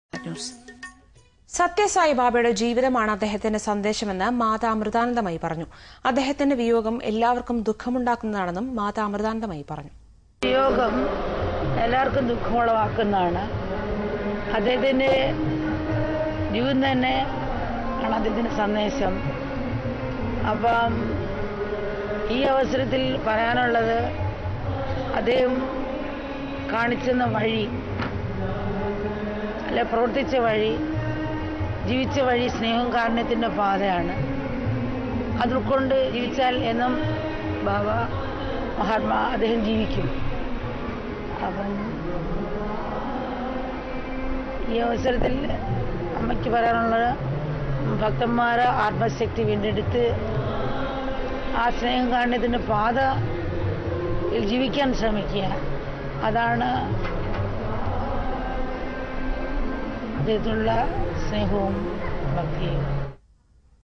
Amma (Matha Amrithanandamayi) Speaks about Sathya Sai baba .EXCLUSIVE ♥♥♥♥
AMMA IS A GREAT SPIRITUAL GURU IN KERALA,INDIA
AND SAI BABA IS SPIRITUAL GURU,MASTER . SPIRITUAL LEADER,GOD IN HUMAN FORM LIKE RAMA AND KRISHNA,JESUS.